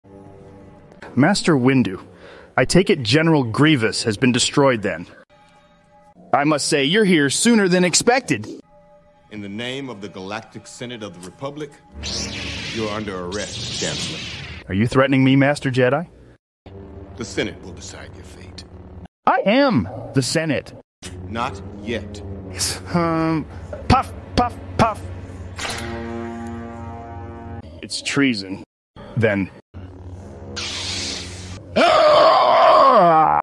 Joe Rogan AI "I AM THE SENATE" Star Wars Meme
This video was made using AI.